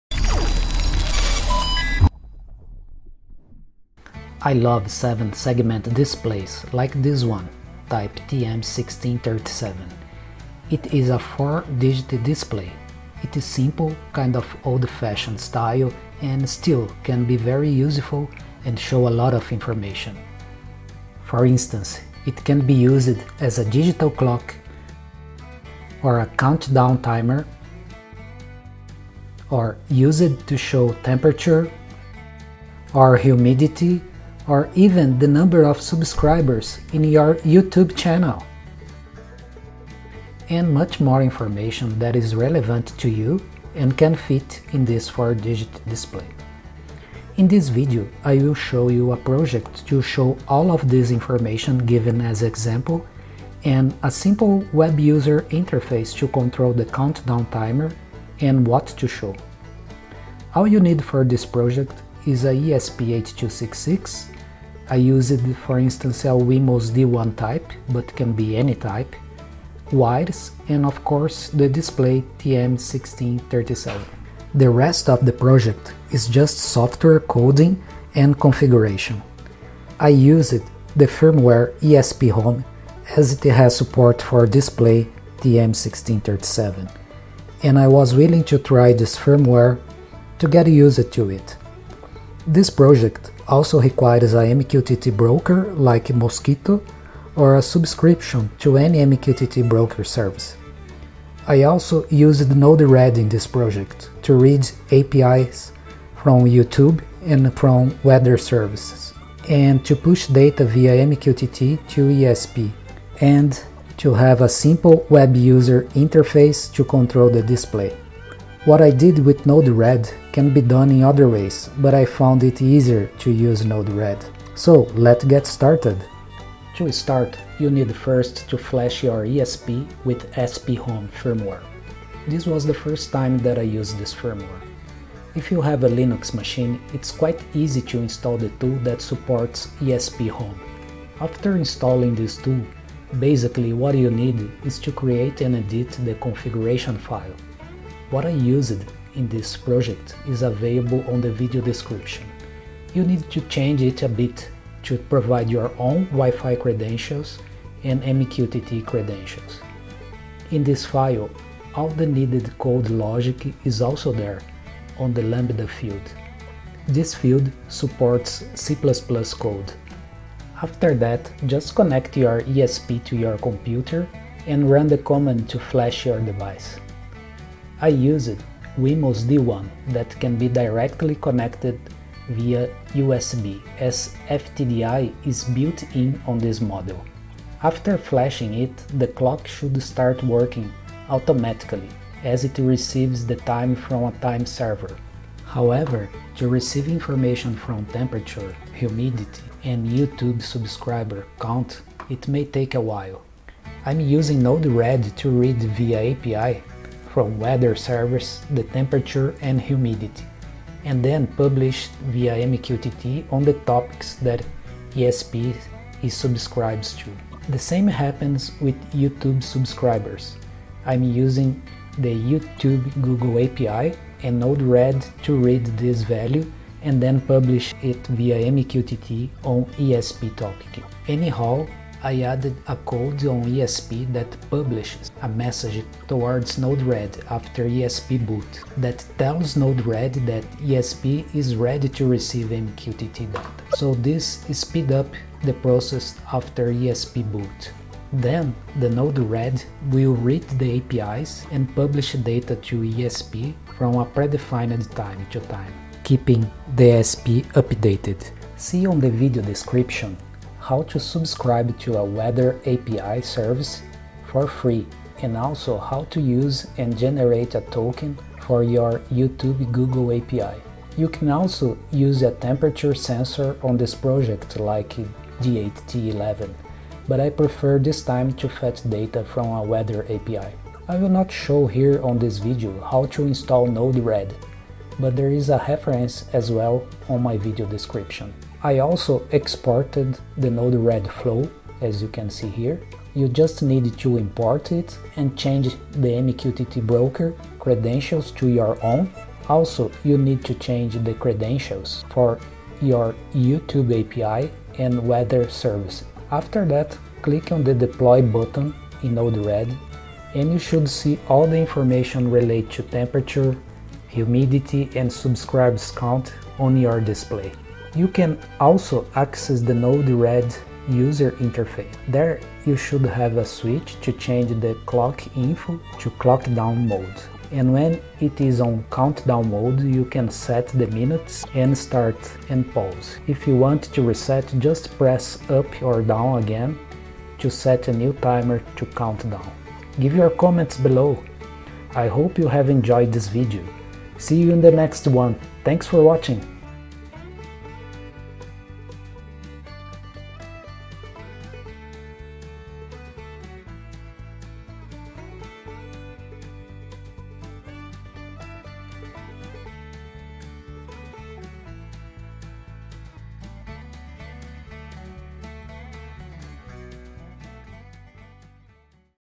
ESPHome on ESP8266 controlling TM1637 display showing Youtube subscribers count and other info
In this video I'm exploring  the 7 segment display type TM1637 controlled by ESP8266 flashed with ESPHome firmware. This 4 digits display is simple and  can be very useful and show a lot of information!

For instance, it can be a digital clock or a countdown timer.
Or
Used to show temperature and humidity

In this video I will show you a project to show all of these information given as example and a simple web user interface to control the countdown timer and what to show.

All you need for this project is a ESP8266 (I used Wemos D1 type, but can be any type), wires and of course the display TM1637.
The rest of the project is just software and configuration.

I used the firmware ESPHome as it has support (library) for display TM1637 and I was willing to try this firmware for the first time.

This project also requires a MQTT broker like Mosquitto or a subscription to any MQTT broker service.

I also used Node-RED in this project to read APIs from Youtube and from Weather service and to push data via MQTT to ESP8266 and to have a simple web interface to control the display. What I did with Node-RED can be done in other ways, but I found it easier to use Node-RED.

In the github link below you will find the ESPHome configuration file that I used. The Node-RED Flow and the 3D printable .stl files.

(You just need to create your account and the free version is enough for this project)

Check this video and give your comments below!